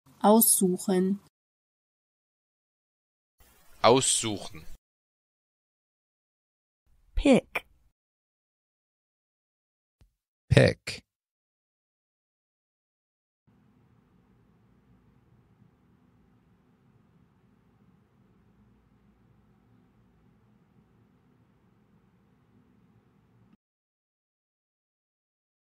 How to say "Pick" in German
Male Voice:
aussuchen

Female Voice:
aussuchen

Male Voice Translation:
Pick

Female Voice Translation:
Pick